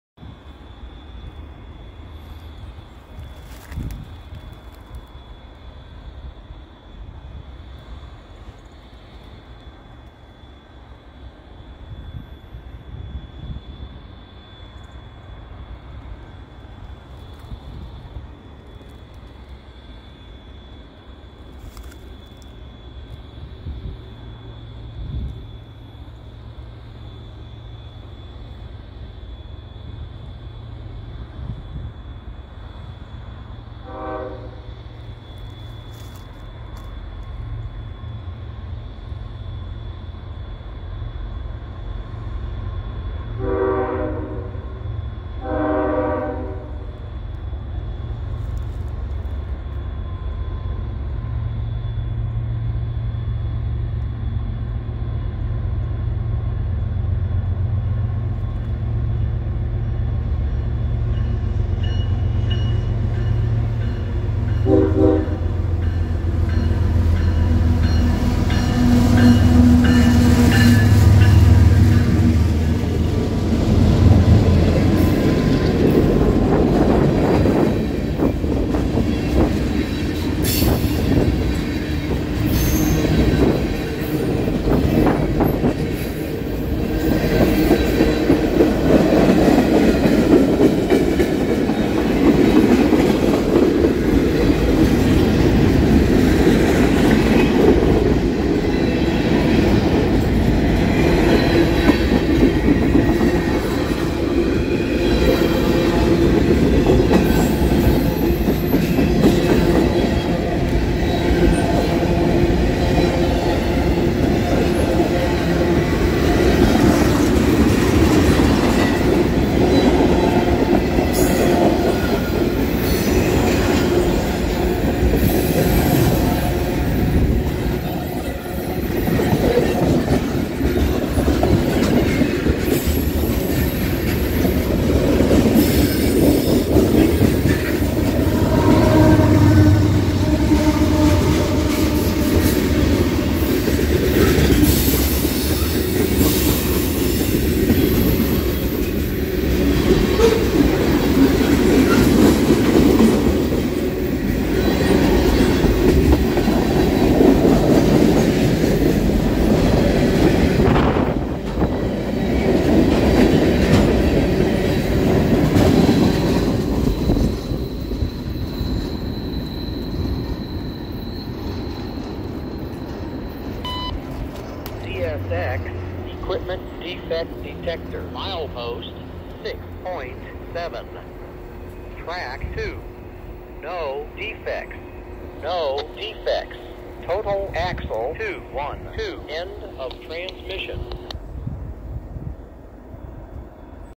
M421 Passes Over Bad Rail At St Denis
CSX 7271 CM44AC DPU(ex CSX 301)
8,800 hp
West 2
3/11/23
3:20 pm